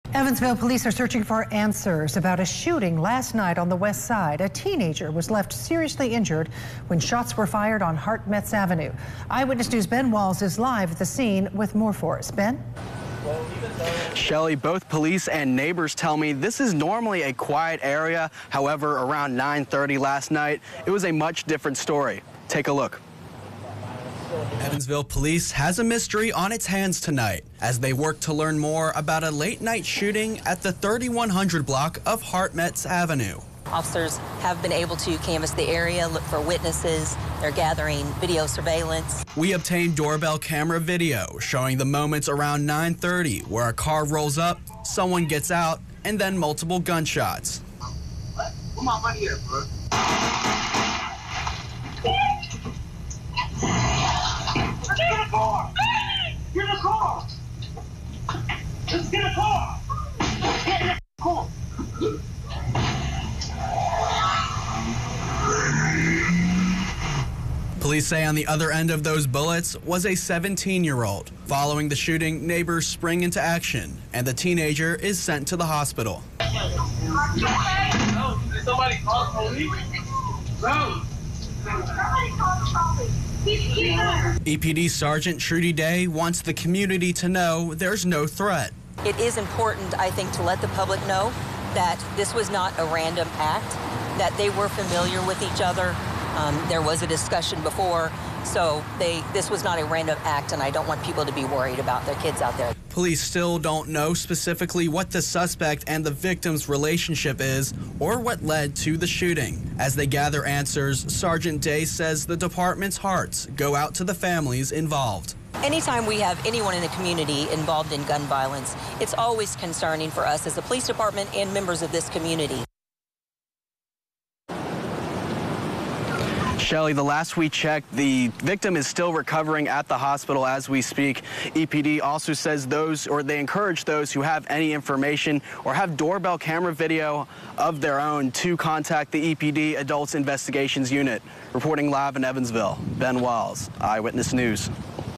EPD investigate west side shooting
One person is injured and the suspect is on the loose following a shooting in west side Evansville.